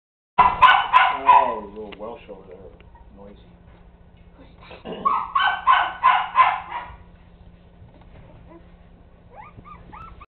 Frenchie's puppies 2nd Visit Part 3
Frenchie's puppies were born on December 31, 2007.  She had 3 boys and 3 girls, please be patient with these clips.  The cage interfered with my camera and then my batteries started to fail.  Subsequent clips are shorter.